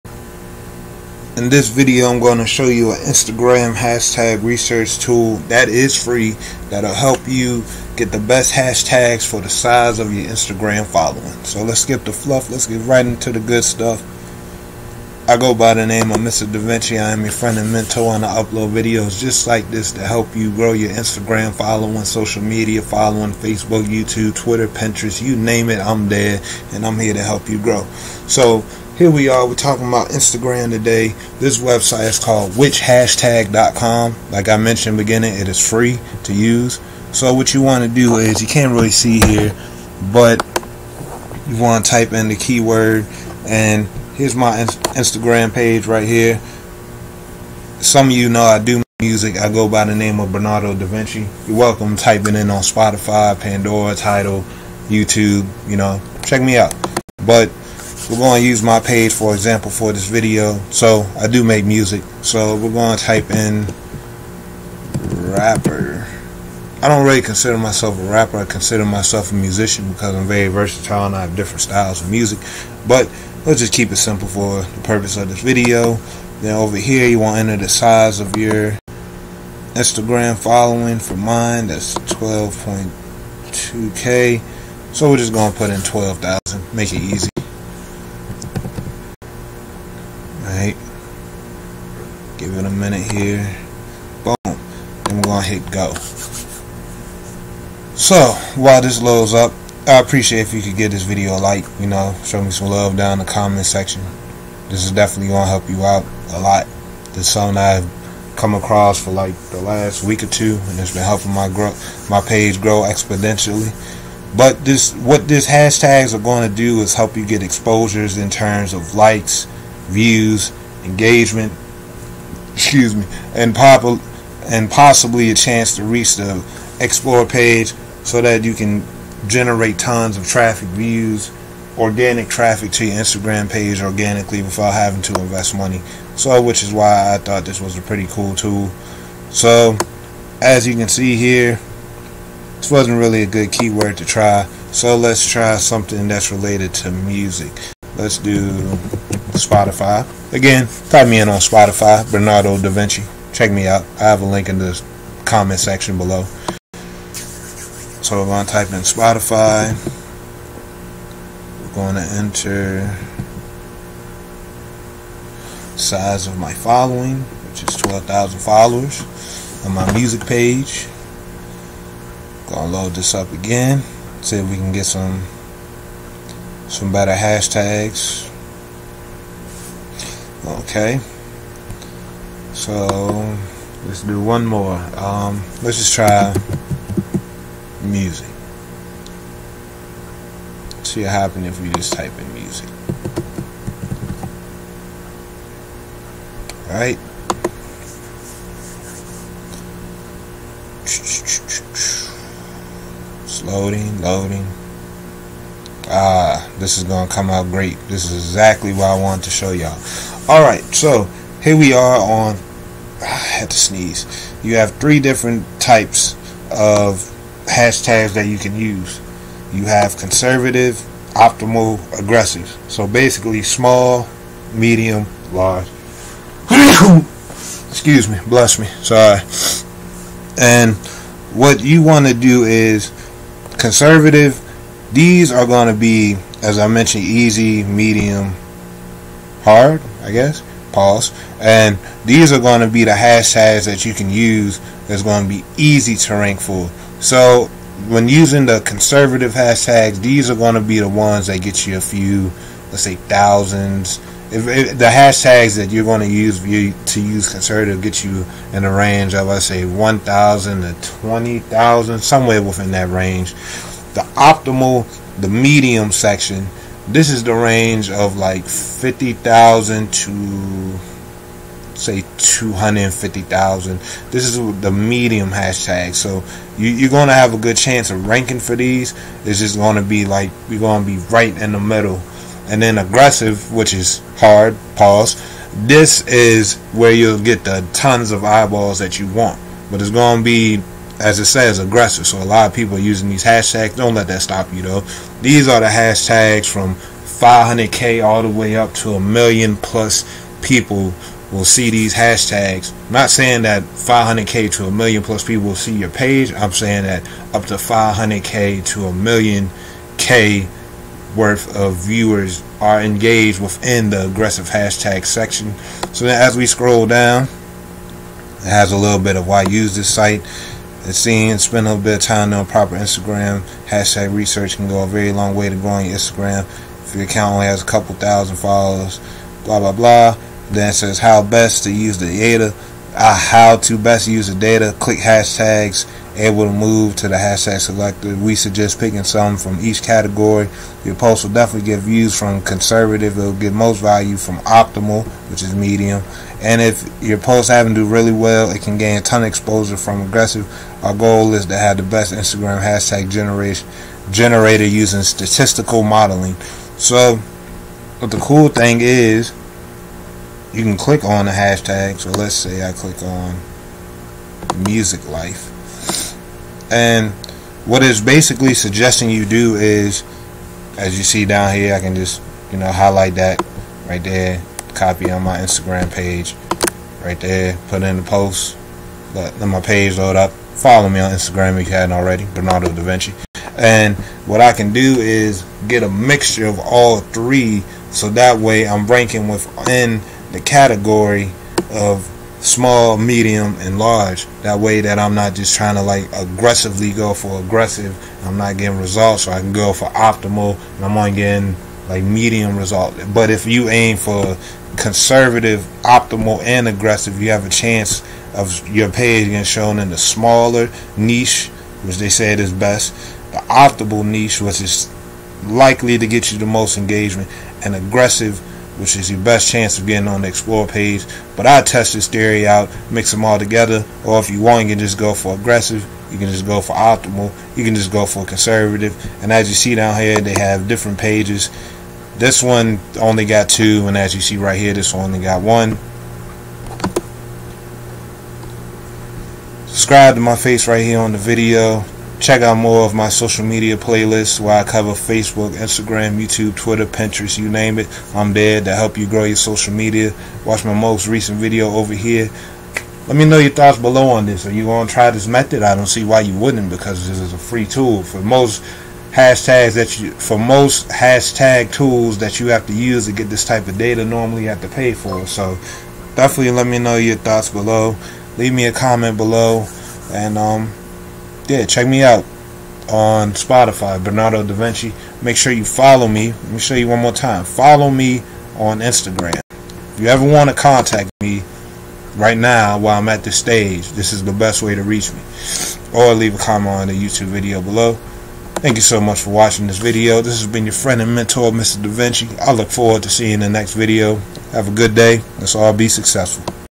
Instagram Hashtag Research 2019
Instagram Hashtag Research Tool. This free hashtag website helps you to get the right size hashtags to use to get more likes and engagements on your Instagram posts.